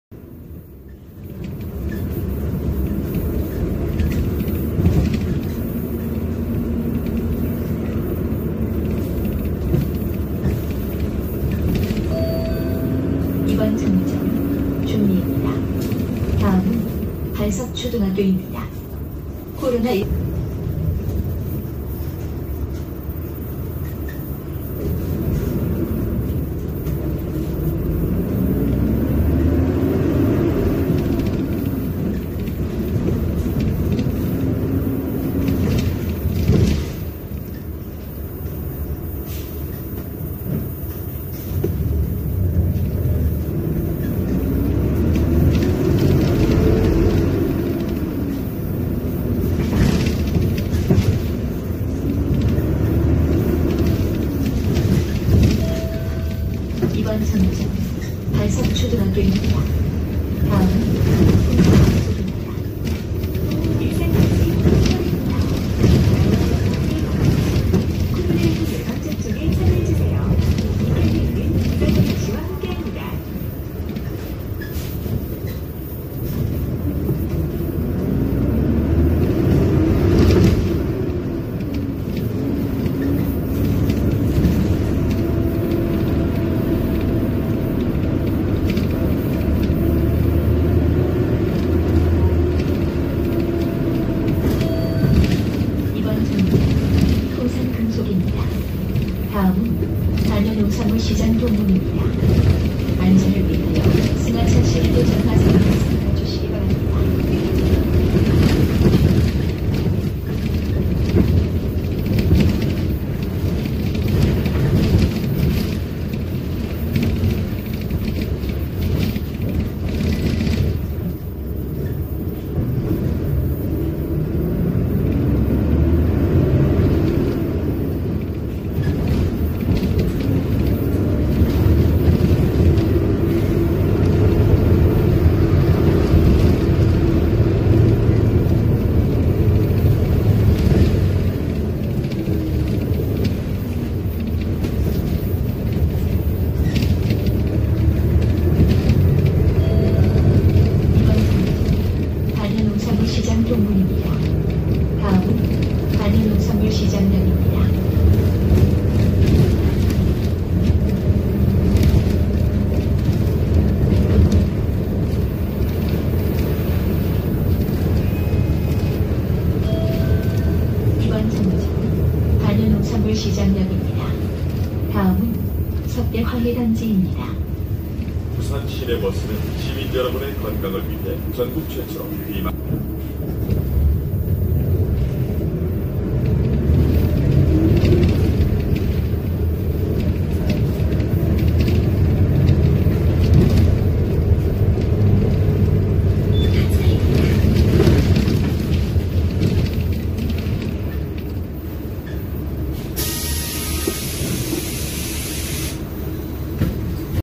[제공영상] 일광여객의 예비차! 4466호 주행영상(촬영:2022/09/20)
일광여객 4466호
동남신버님의 제공영상입니다.
일광여객에서 대규모 신차가 나오면서 31번에서 격하된 4466호..
이 차량 4464호와 더불어 상태가 안 좋기로 유명한데 마침 동남신버님께서 타셔서 끼리릭부분을 제공해주셨습니다!
전 31뉴슈퍼 4465호를 타서 4464도 4466도 본 적 없지만 상태는 진짜 체감이 되네요..
제공해주신 동남신버님 감사드립니다.